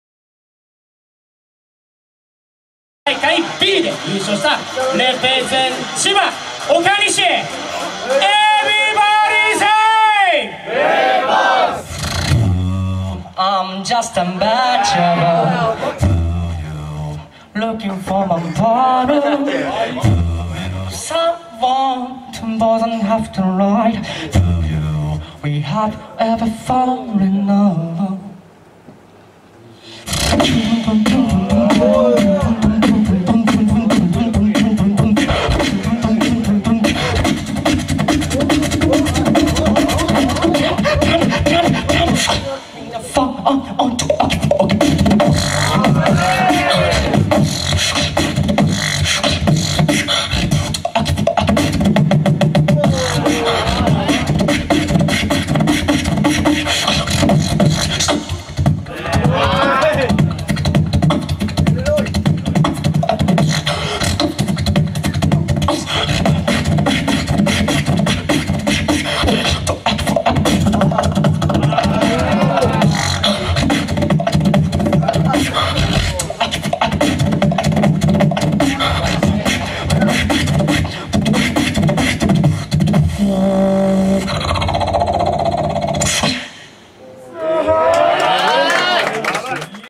Grand Boost Championship 本予選 WOKANISHI
2017 .1/7
赤坂TENJIKU

WOKANISHI
レペゼン千葉
東京大会バトルB優勝

MC：AB-BOY バハムート
JUDGE：AFRA / TATSUYA

プレイヤーだけではなくスタッフ、オーガナイザー、ジャッジなど全ての若手ビートボク­サーを応援するプロジェクトイベント「Boost」